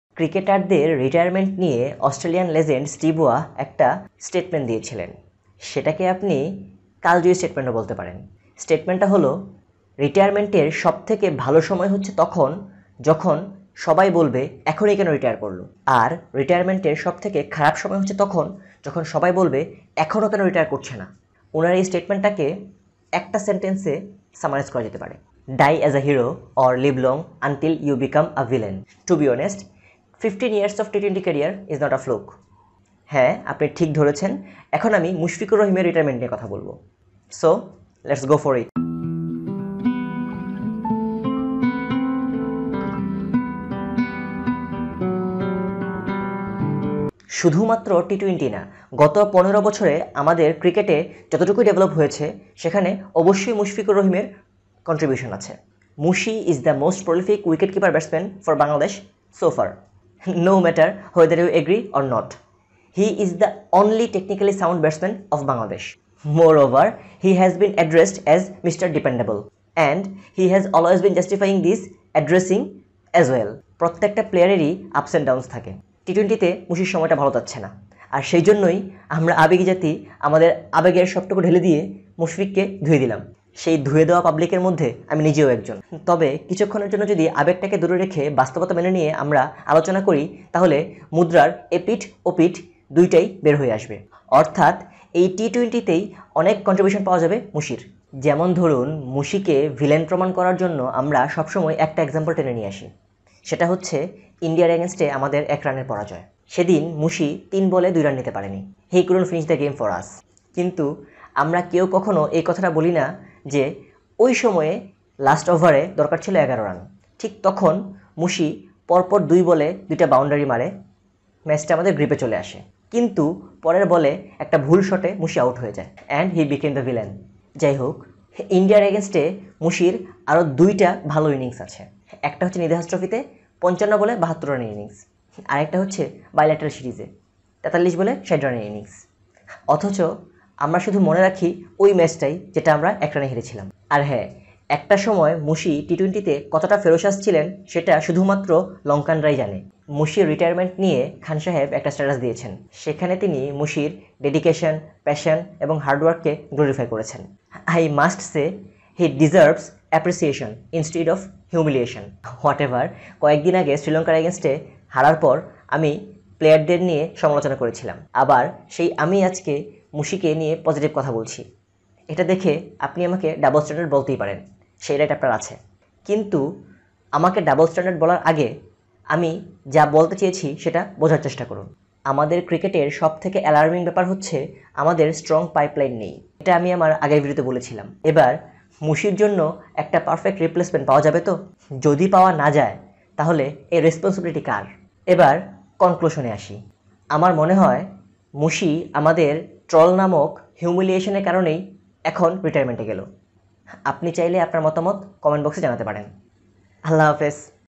মুশফিকের অবসর ǀ Mushfiqur Rahim ǀ Cricket ǀ Prudential Passion ǀ T20 ǀ Bangladesh ǀ BCB ǀ TRP
Mushfiqur Rahim is the most prolific wicket keeper batsman of Bangladesh so far. He has been outstanding throughout his career. Recently he is out of form in T20 format, and we’re the crazy fans making fun of him for his poor performance. His T20 career is longer than 15 years and it can’t be a fluke anyway. He might get hurt and announced his retirement from T20.
TBH, we don’t have the perfect replacement of Mushi as we don’t have a strong pipeline of players. And BCB has to take this responsibility.
BTW, it’s not an expert opinion. It’s a personal opinion of a passionate fan of Bangladesh Cricket Team.
Best of luck, Mushi. Best of luck, TeamTigers.